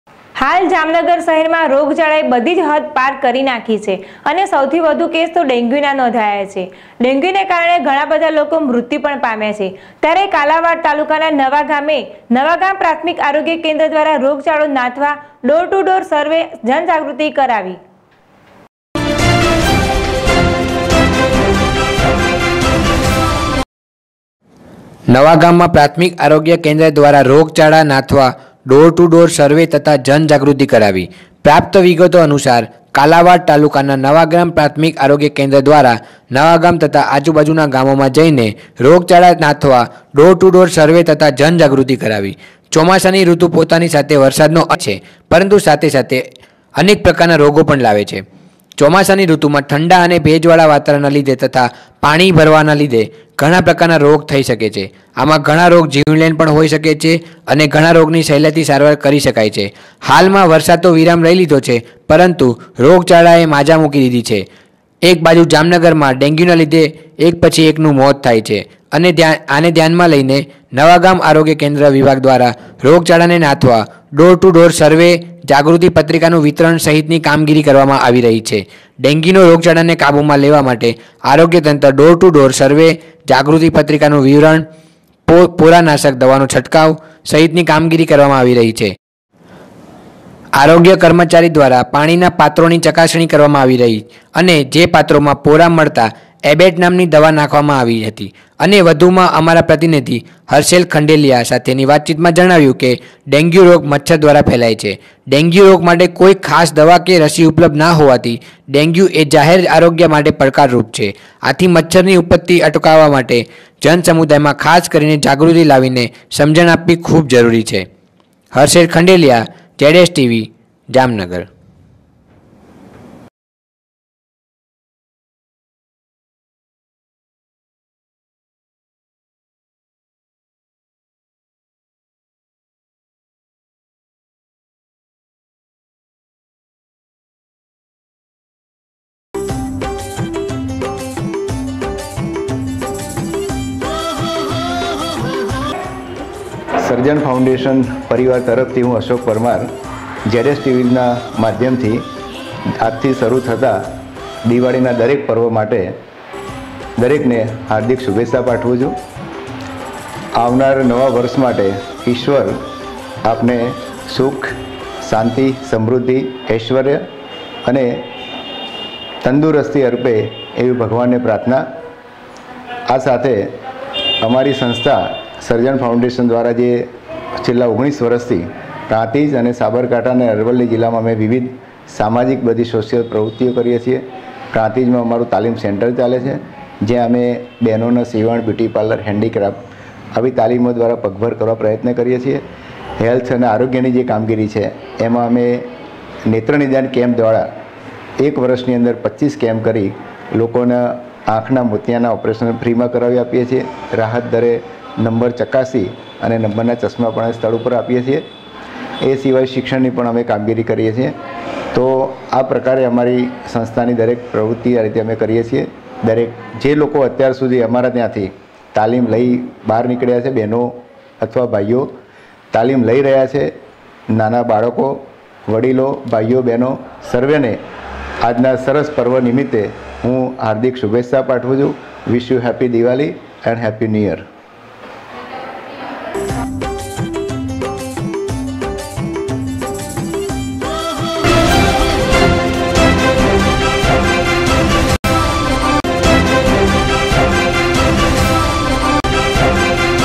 હાલ જામનગર શહેરમાં રોગચાળાએ બધી હદ પાર કરી છે //ZSTVNEWS//
હાલ જામનગર શહેરમાં રોગચાળાએ બધી હદ પાર કરી છે અને સૌથી વધુ ડેંગ્યુના કેસો નોંધાયા છે અને ડેંગ્યુને કારણે ધણાં બધા લોકો મૃત્યુ પામ્યા છે ત્યારે કાલાવડ તાલુકાના નવાગામે નવાગામ પ્રાથમિક આરોગ્ય કેન્દ્ર દ્ધારા રોગચાળો નાથવા ડોર ટુ ડોર સર્વે તથા જન જાગૃતિ કરાવી.